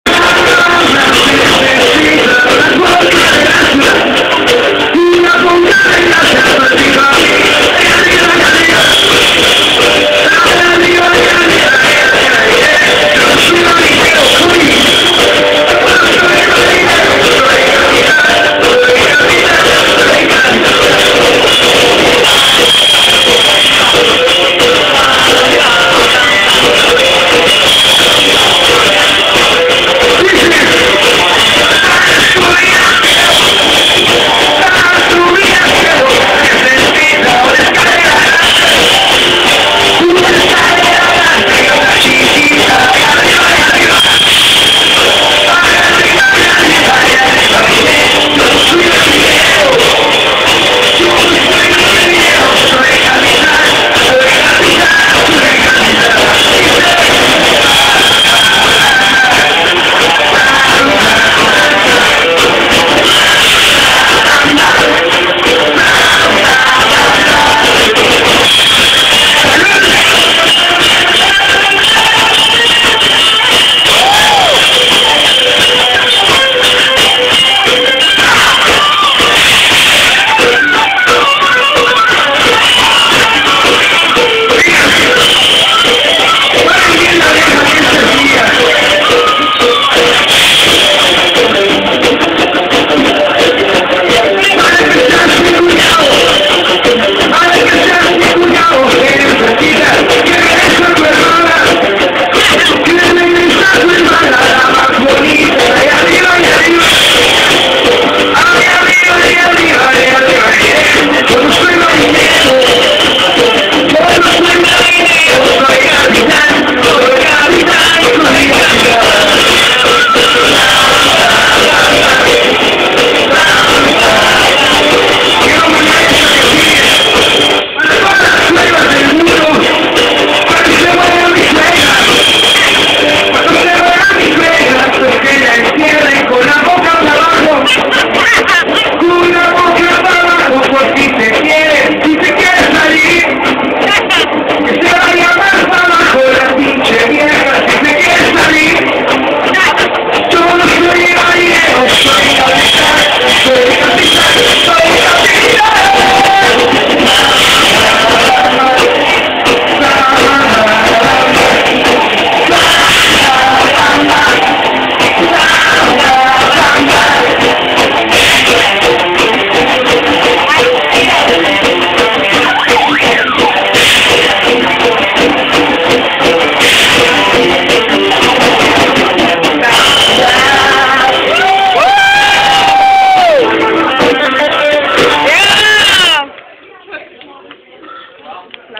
HAVING FUN AT CLUB03.3GP
having fun at club listneing to one of our local groups sing.